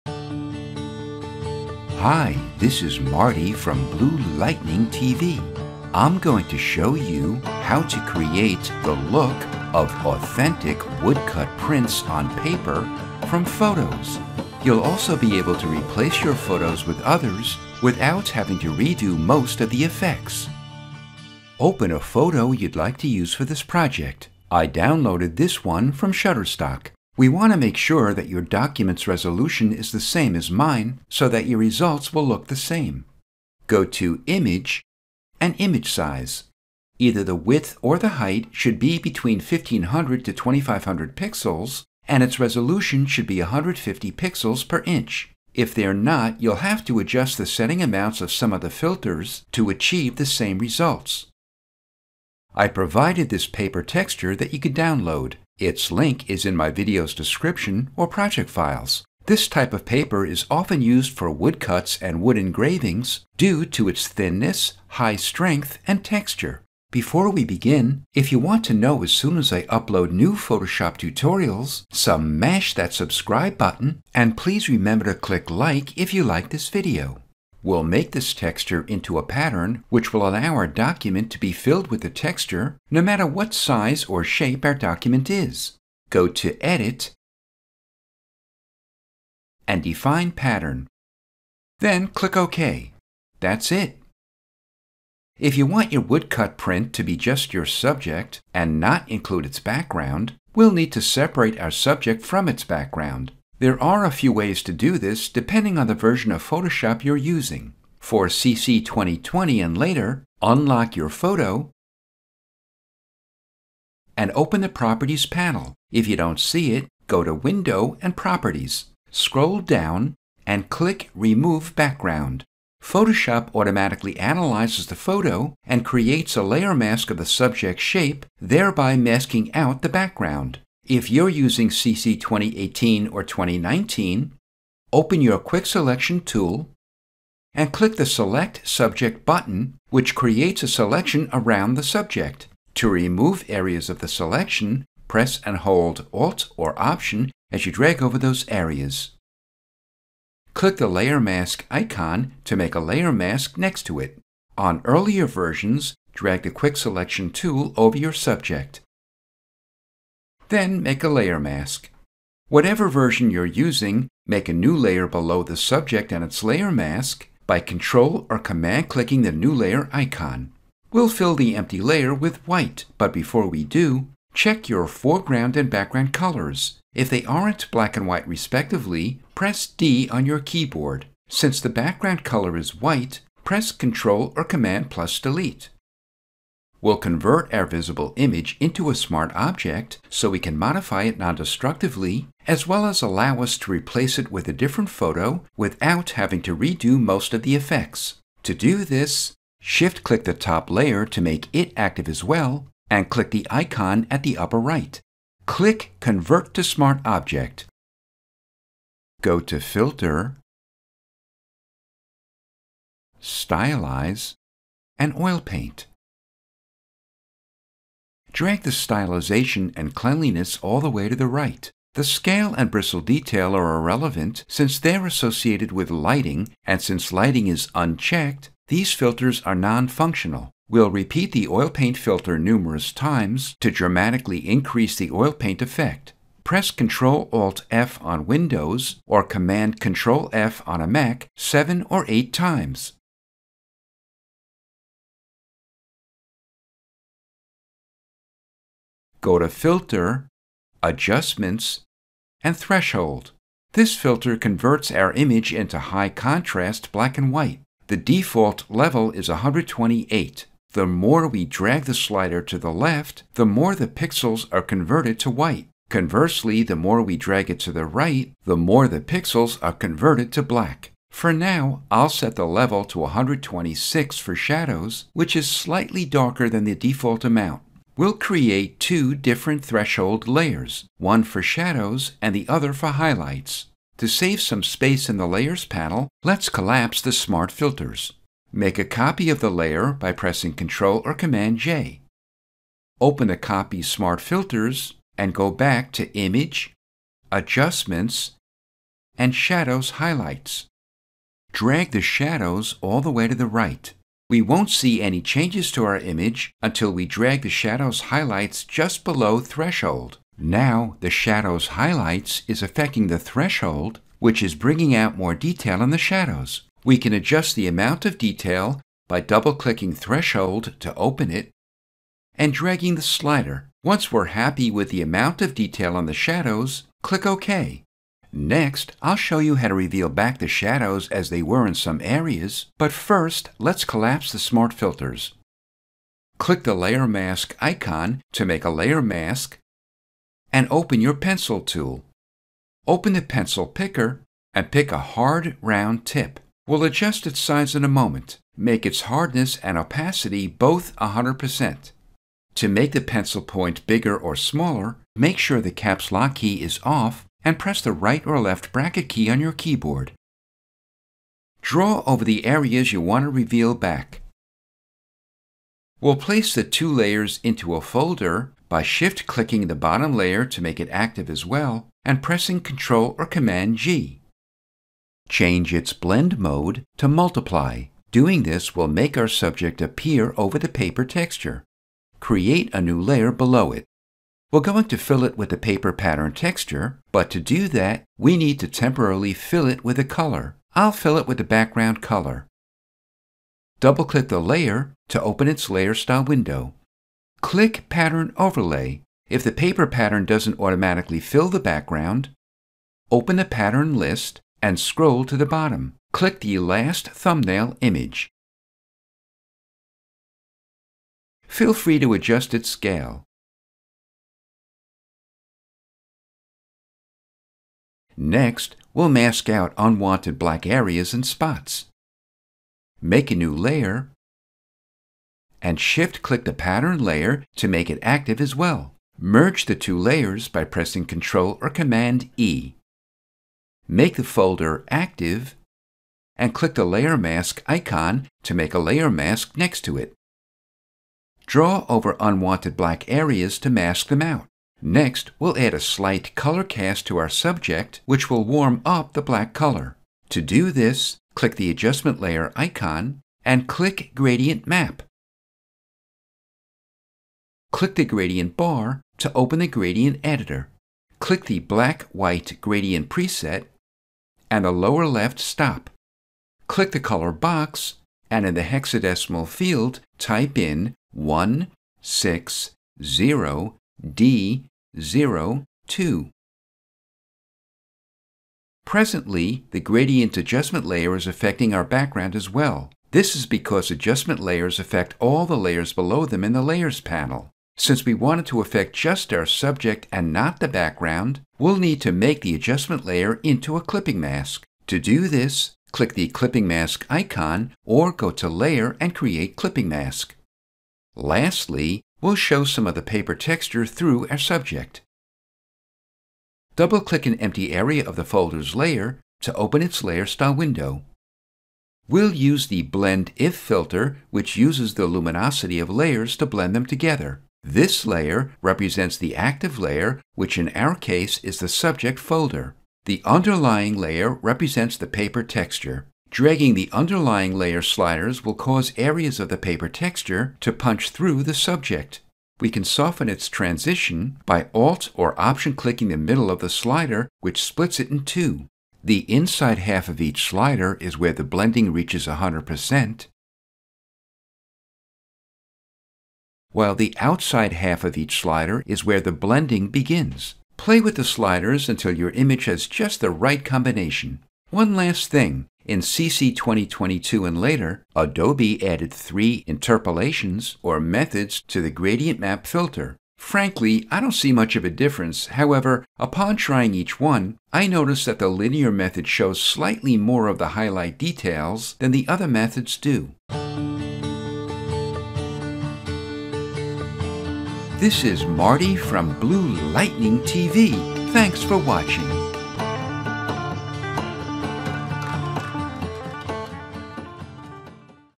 Photoshop: Create the Look of a WOODCUT Print Portrait!
Photoshop CC 2022 tutorial showing how to recreate the look of an authentic, woodcut print on paper.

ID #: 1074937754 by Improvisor
ID #: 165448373 By Eric Isselee

ID #: l_56442995

BLTV MERCH @ Redbubble:  BLTV.redbubble.com